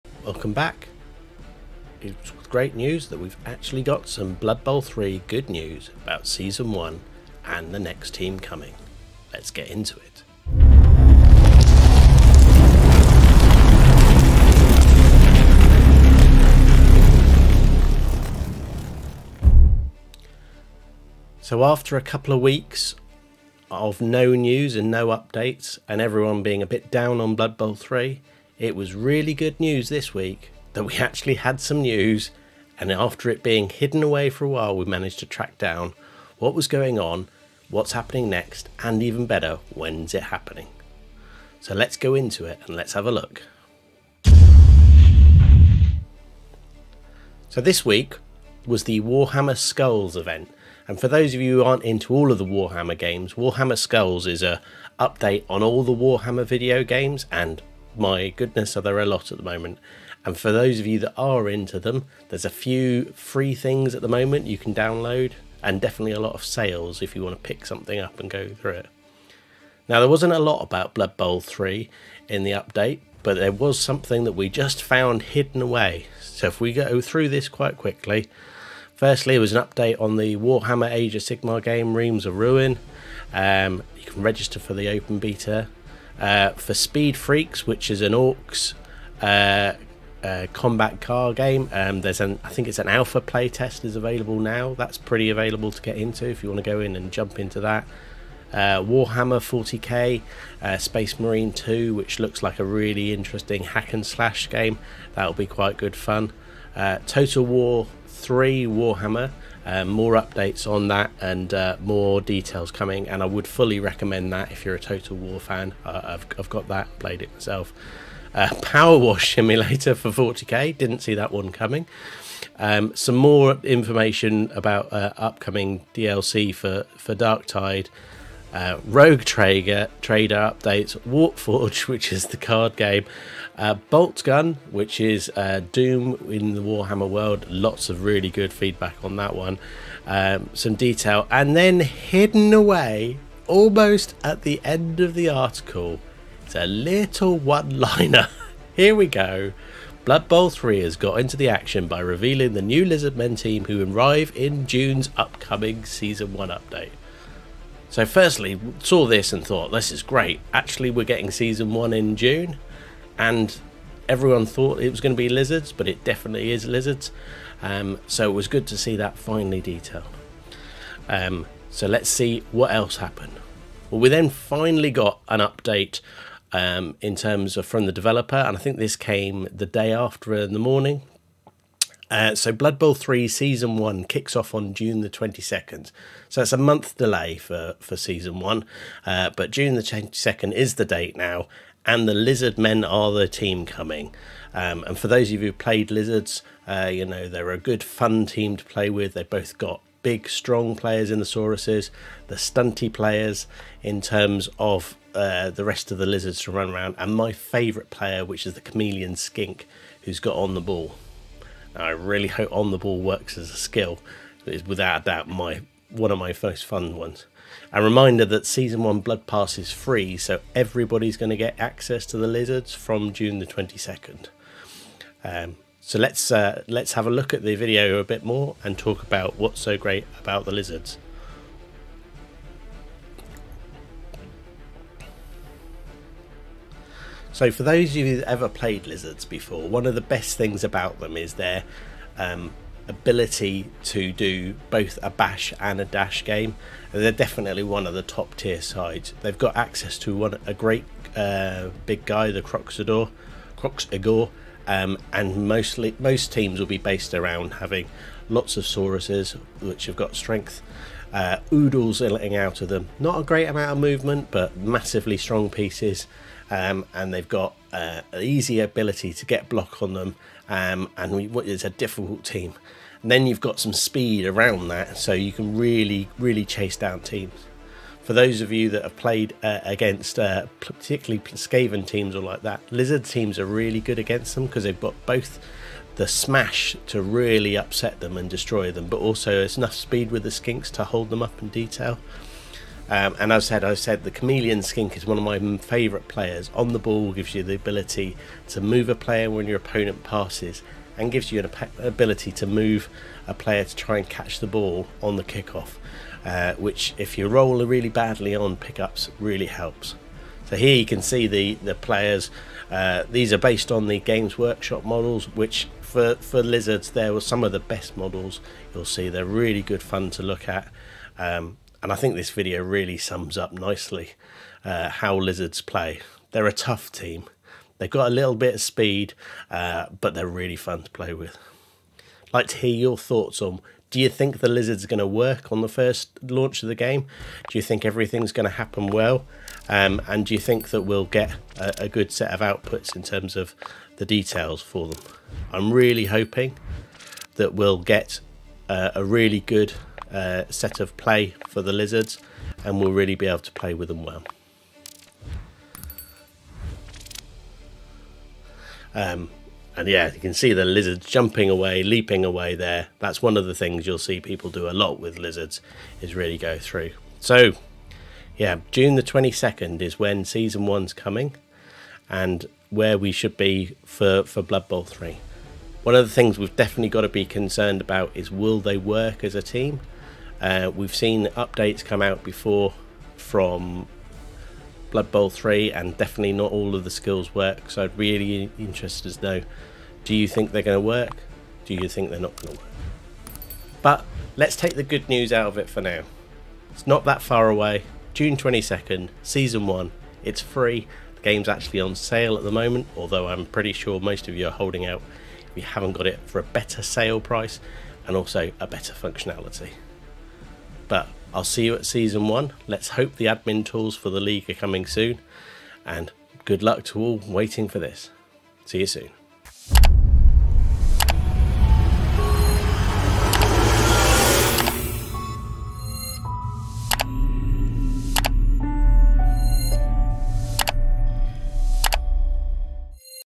Blood Bowl 3 Season 1 date and team reveal
There's been an update on Season 1 of Blood Bowl 3 - and the date and the new team have been revealed! Lets have a look and maybe get excited?

Remove, replace and blur your background without a Green Screen?